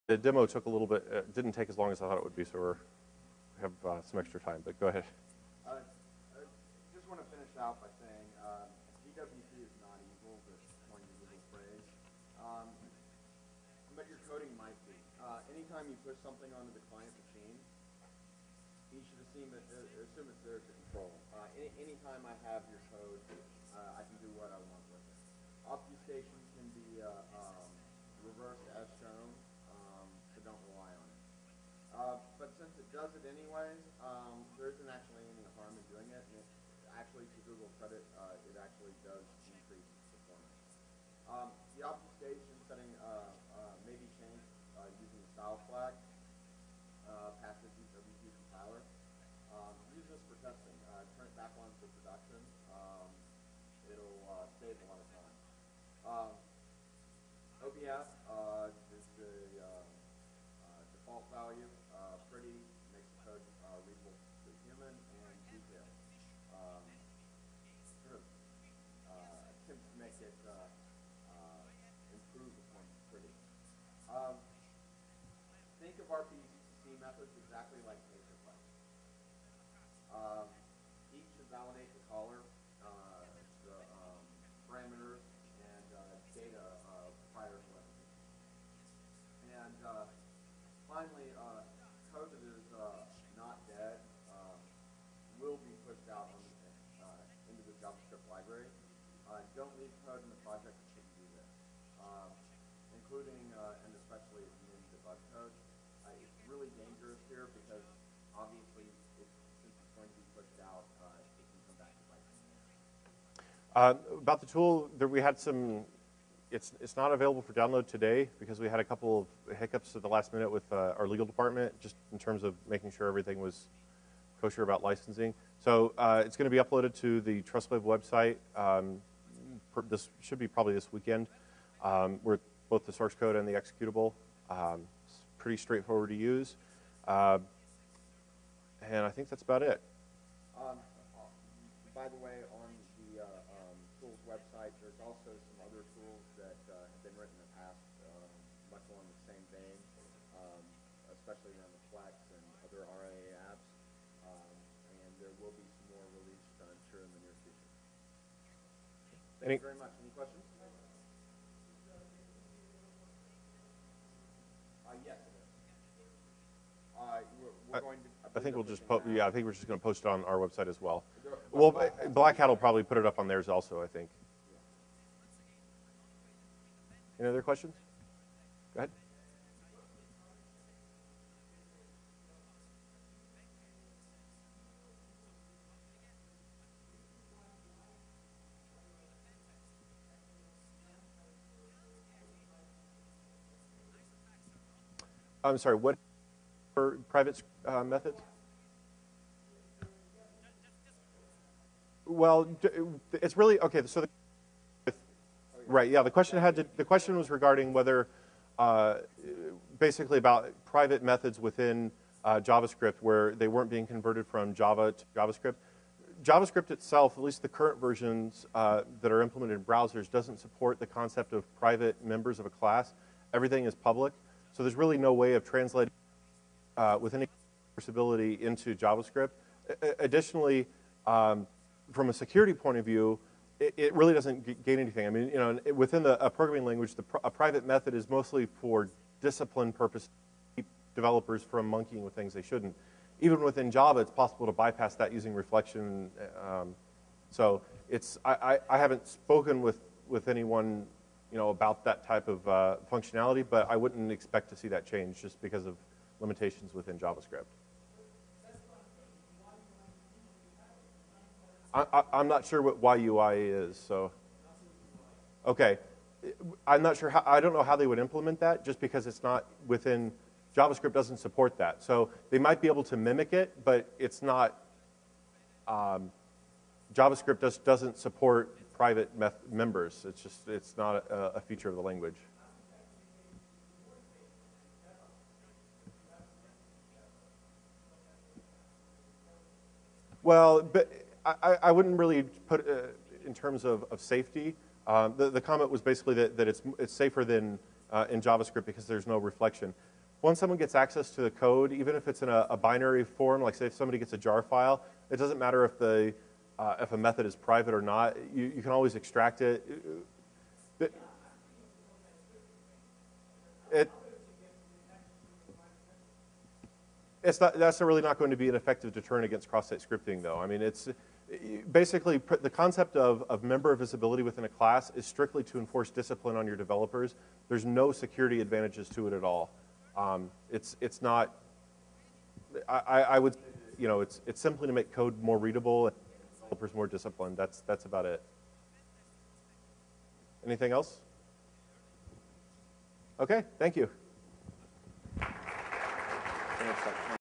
Black Hat USA 2010: GWT Security: Don't Get Distracted By Bright Shiny Objects 4/4
Speakers: David Byrne, Charles Henderson

The Google Web Toolkit (GWT) produces some of the slickest web-based applications out there; it's easy to understand why it has been gathering popularity. But there's obviously more to writing a secure application than making the UI nice and shiny. The framework's functionality is not limited to GUI controls - it also has significant support for remote procedure calls. The browser-side code is implemented entirely in JavaScript that the GWT generates from developer-written Java code. While GWT RPC can be implemented securely, many developers either rely on the JavaScript obfuscation, or don't realize how their Java code is going to be split between the server & client. Either way, insecure GWT remoting is very common.

This presentation will demonstrate how to exploit common vulnerabilities in GWT applications, particularly with RPC functionality. Non-human readable format of the JavaScript makes penetration testing GWT applications very time consuming. To aid with testing, the presenters are releasing REGWT, a tool to reverse engineer GWT applications. It will allow a pen tester to map out GWT RPC methods that would otherwise be hidden and easily test them for various vulnerabilities.